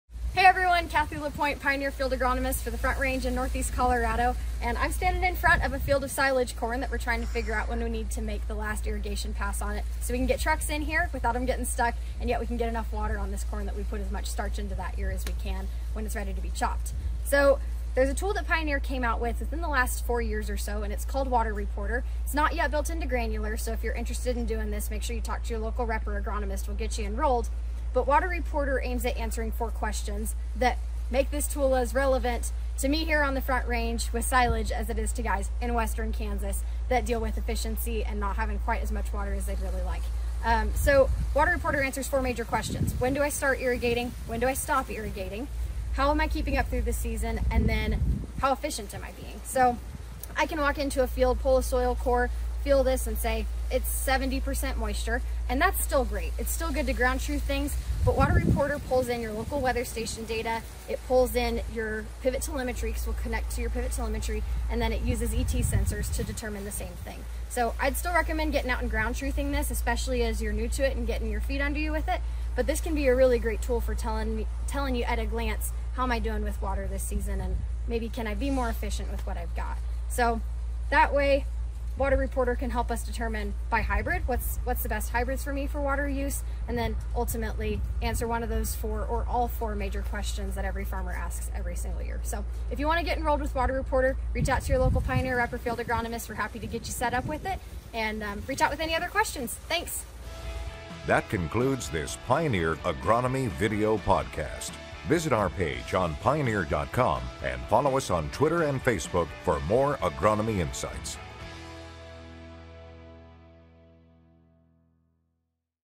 What Is Water Reporter?
💧 One of the most important metrics to monitor in the field is moisture. 💦

With Pioneer's new Water Reporter tool, see data from the local weather services as it specifically applies to your fields. Kathi LaPoint, a Pioneer field agronomist from Colorado, explains how it works and what questions it can help answer during this video. Contact your local Pioneer rep to enroll! 🌽